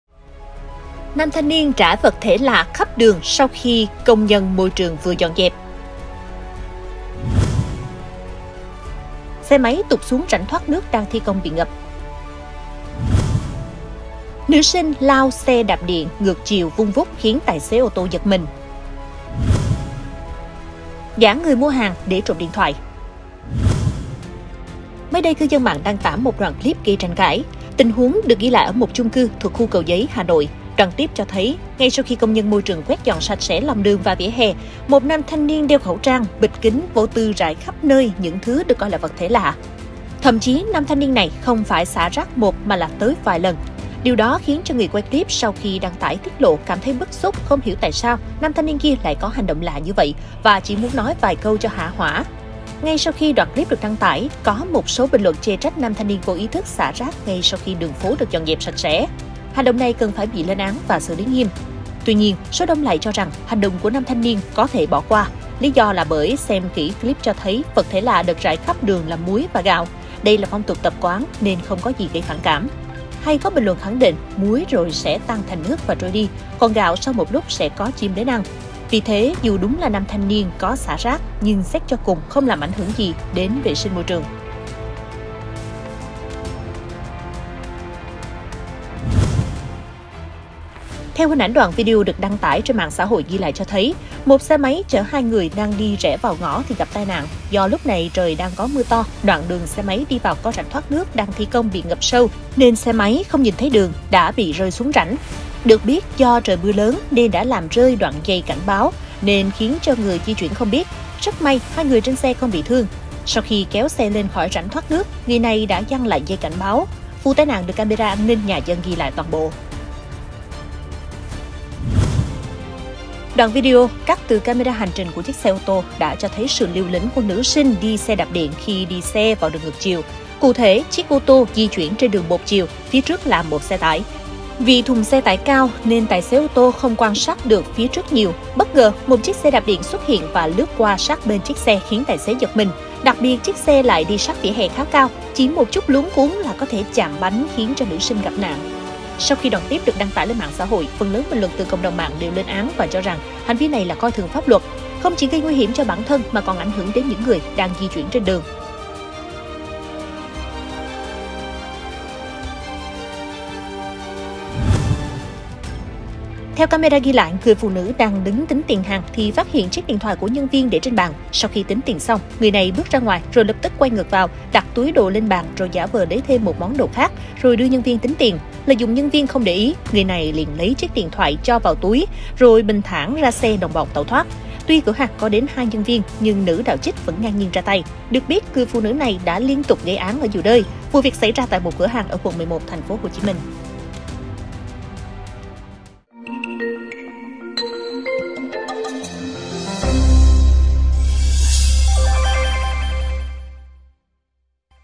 Nam thanh niên rải 'VẬT THỂ LẠ' khắp đường sau khi công nhân môi trường VỪA DỌN DẸP
Những đoạn clip camera nổi bật mỗi ngày:
00:00 LIST CAMERA
00:24 NAM THANH NIÊN RẢI ‘VẬT THỂ LẠ’ KHẮP ĐƯỜNG SAU KHI CÔNG NHÂN MÔI TRƯỜNG VỪA DỌN DẸP
01:30 XE MÁY TỤT XUỐNG RÃNH THOÁT NƯỚC ĐANG THI CÔNG BỊ NGẬP
02:03 NỮ SINH LAO XE ĐẠP ĐIỆN NGƯỢC CHIỀU VUN VÚT KHIẾN TÀI XẾ Ô TÔ GIẬT MÌNH
02:45 GIẢ NGƯỜI MUA HÀNG ĐỂ TRỘM ĐIỆN THOẠI

saigontvofficial Xem thêm thông tin và theo dõi chúng tôi tại